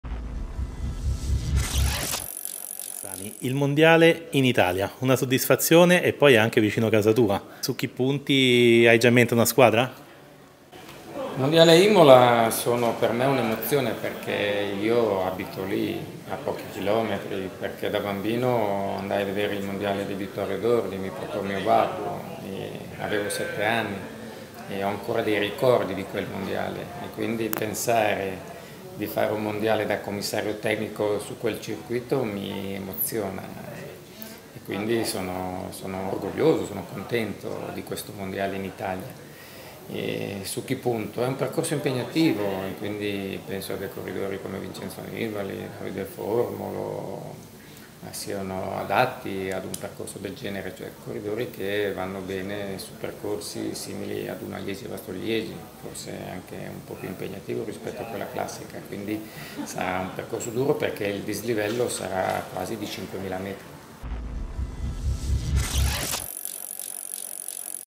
Davide Cassani e il Mondiale a Imola
Cassani parla del Mondiale di Ciclismo su Strada a Imola: